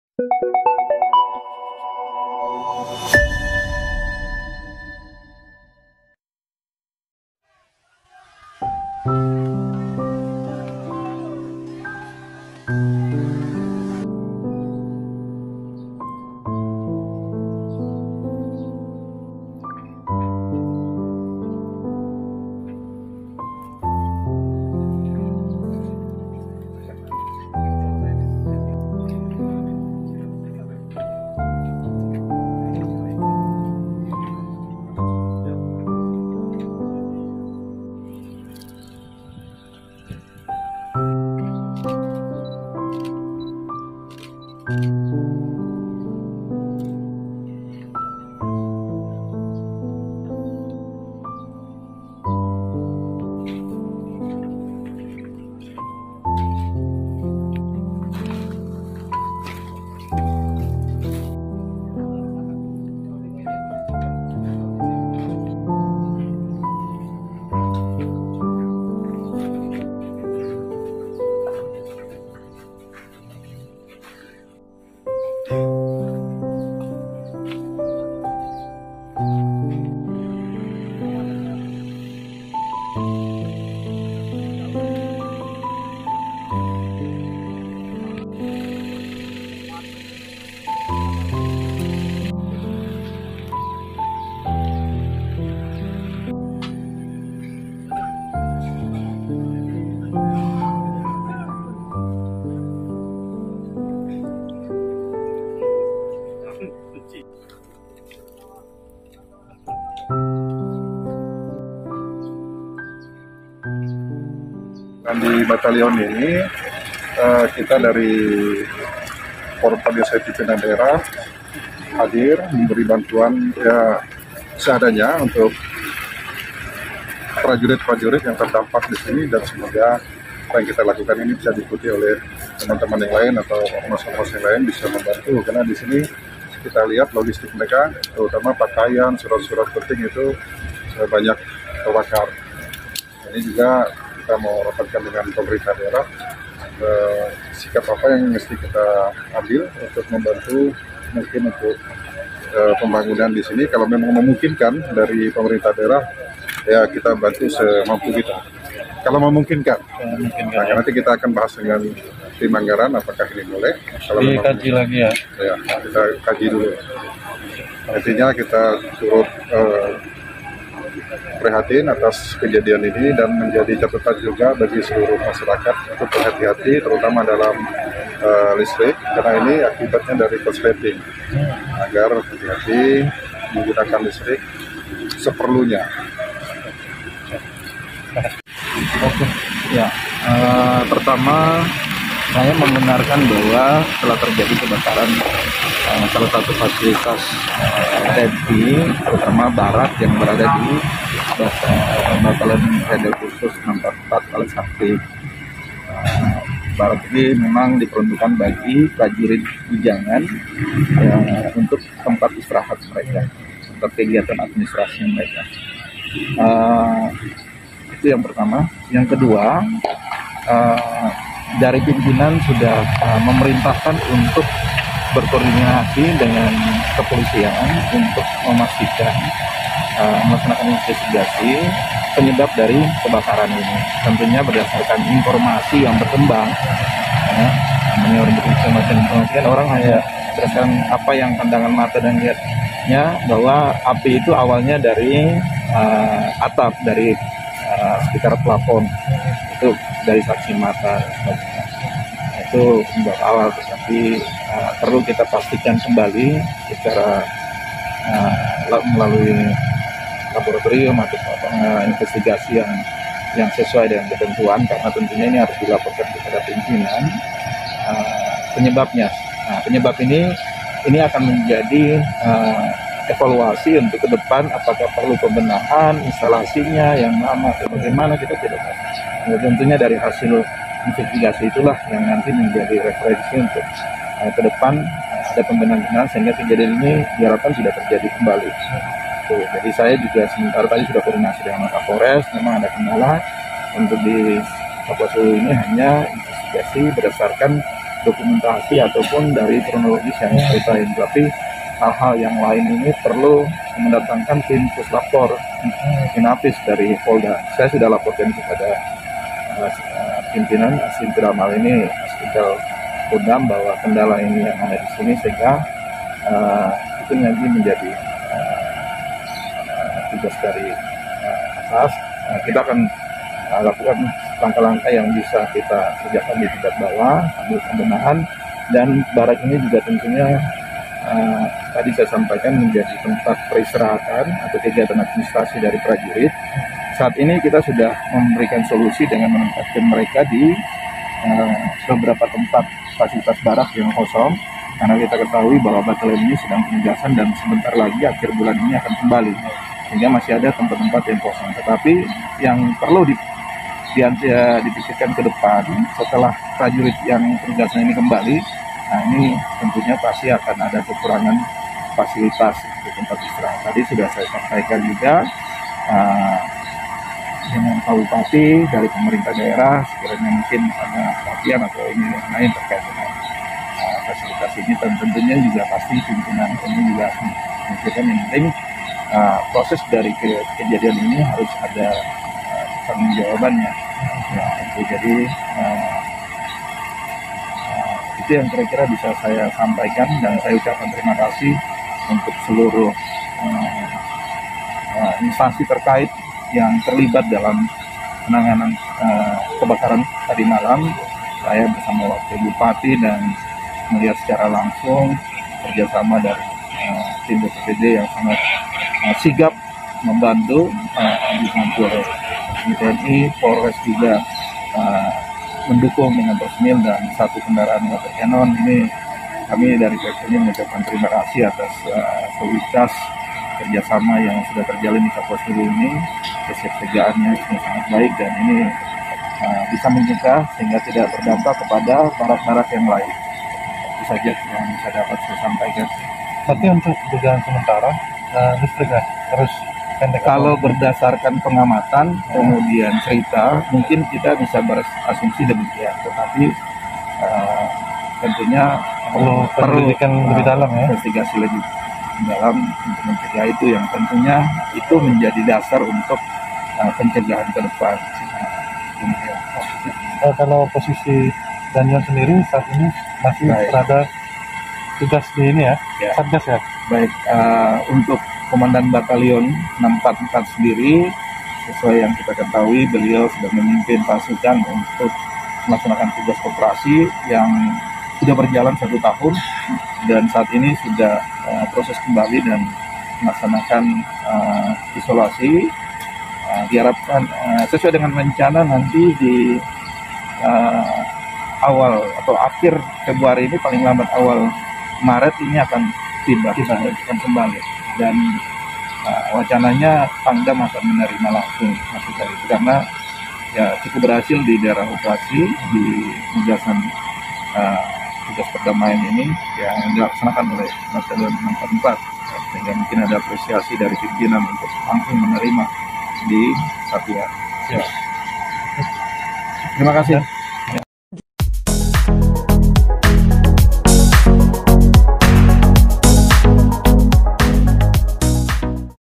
Kunjungan Pasca Kebakaran Barak Batalyon Infanteri Rider Khusus 644 Walet Sakti
Kobaran api menghanguskan salah satu barak di Mayonif RK 644/Wls, Kapuas Hulu, Kalimantan Barat, Rabu (9/2/2022). Pasca kebakaran, Bupati Kapuas Hulu, Fransiskus Diaan bersama Forkopimda setempat mengunjungi lokasi kebakaran Kamis (10/2/2022).